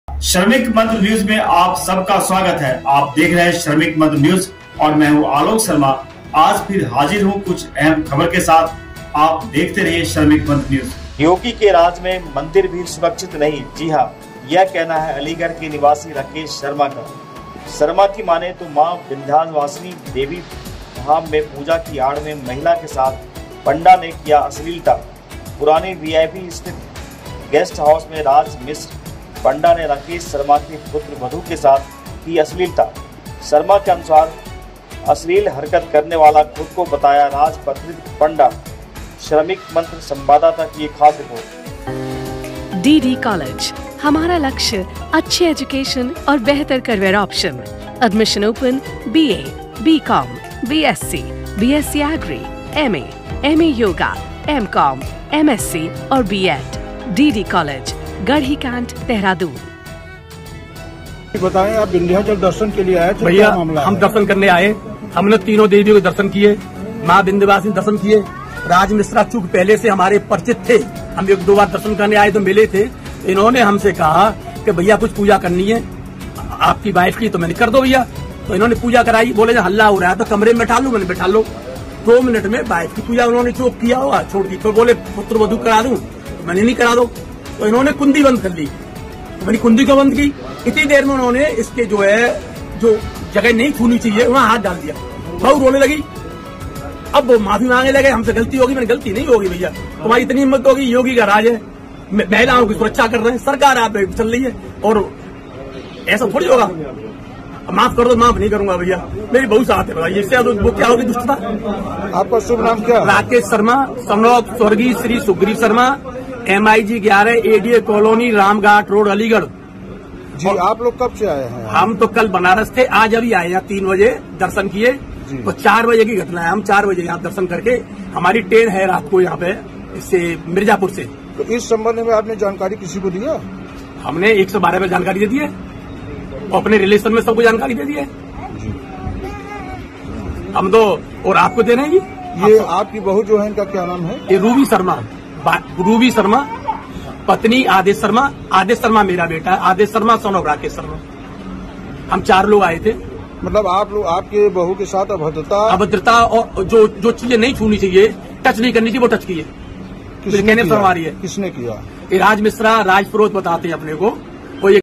मंदिर के पुजारी ने किया अश्लीलता : राकेश शर्मा,पीड़ित
राकेश शर्मा के पुत्र वधू के साथ मंदिर के पंडा ने की अश्लीलता